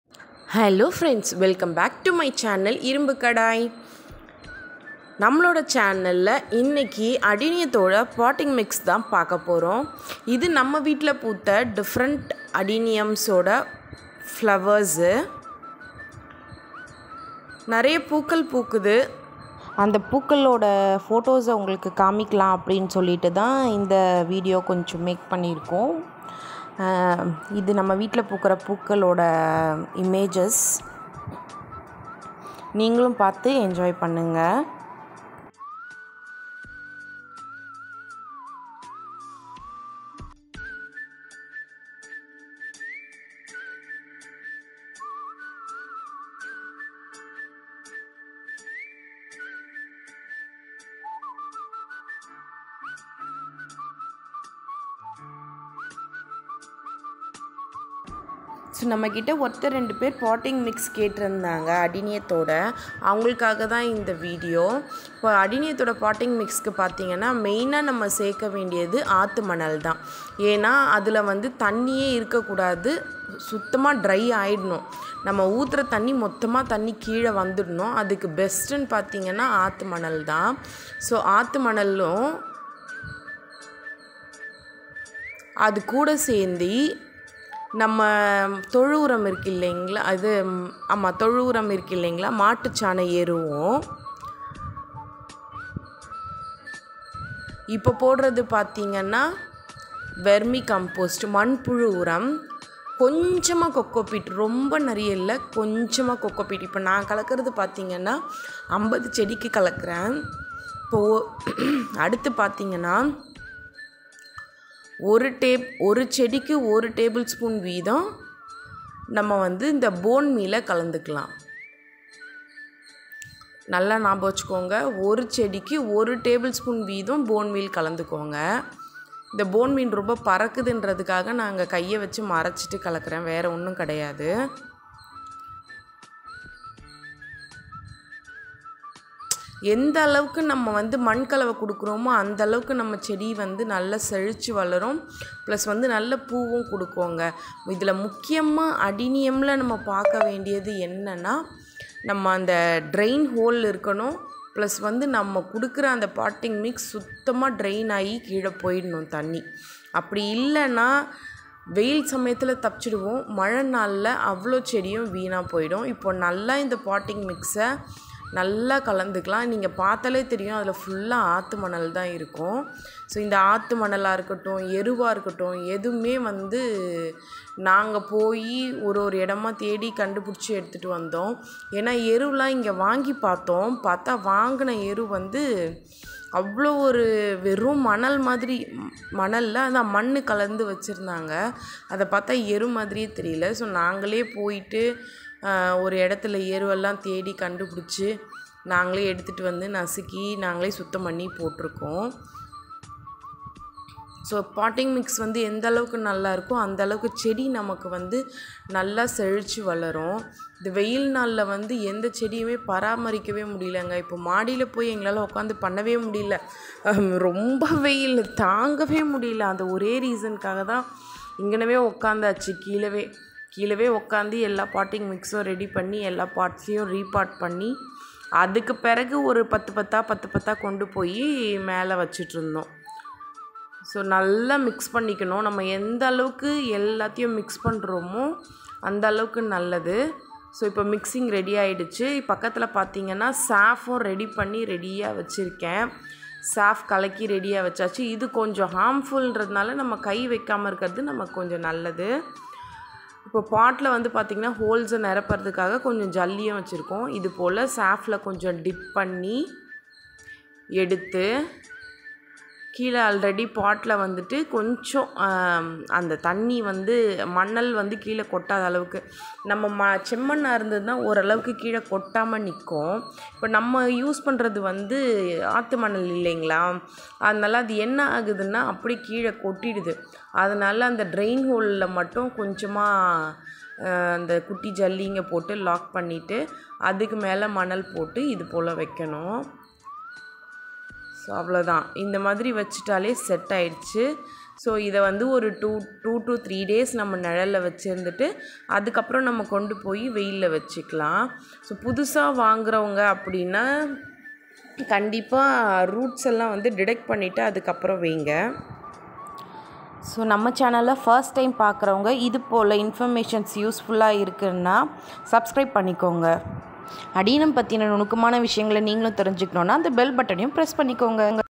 அடினியம் மலர் கண்காட்சி 💥அடினியம் மண் கலவை 🧐
என் வீட்டில் உள்ள அடினியம் பூக்களை உங்களுக்கு காண்பிக்க மலர் காட்சியை இணைத்துள்ளேன்.
     அடி னியத்தின் மண் கலவை  மற்றும் உரம்